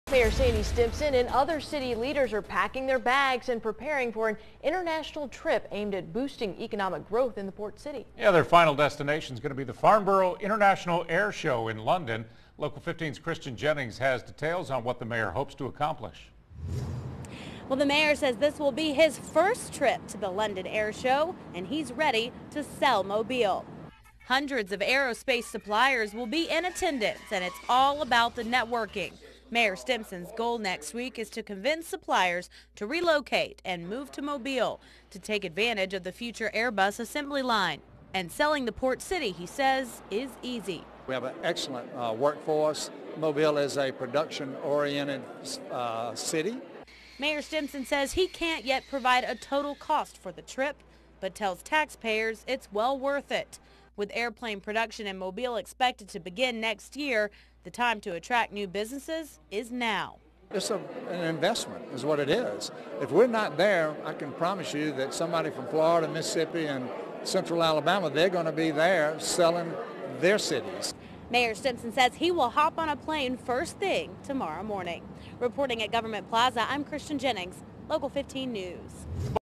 State and Local Leaders Going to Airshow
Local and state leaders will be heading to England this weekend for the Airbus sponsored Farnborough International Airshow.

Alabama Congressman Bradley Byrne and Governor Robert Bentley will be at the event. Also making the trip will be Mobile Mayor Sandy Stimpson, several Mobile City Council  members and Mobile County Commissioners.

County Commissioner Connie Hudson joined Local 15 Today Thursday during the 6:30 a.m. half hour to talk about the trip. Watch the video for more information.